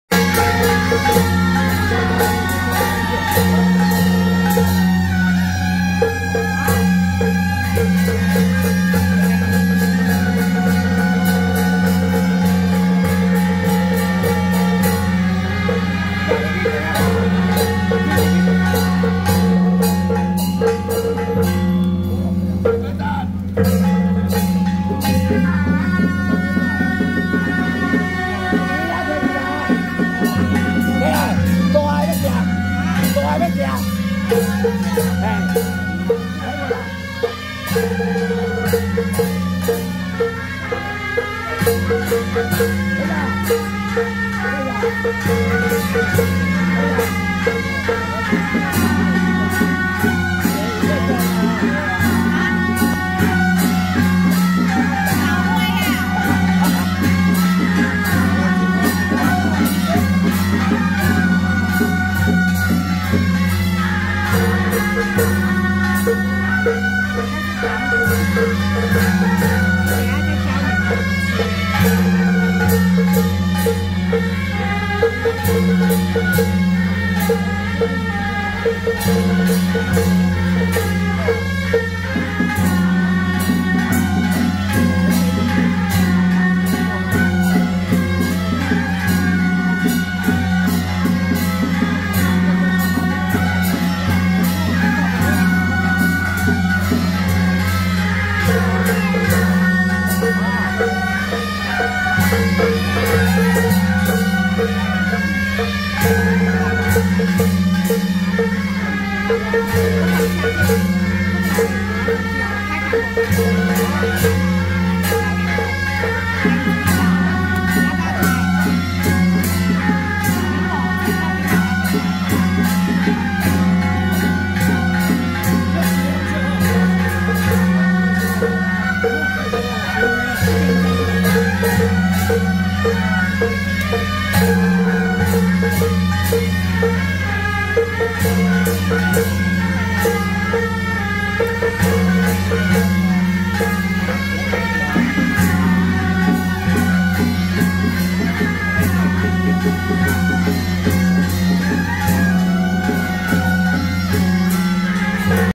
【景美景興社-神將團】~112景美景興太子宮景興媽南巡謁祖進香回駕遶境
桃園聖安社-北管＆景美景興社-神將團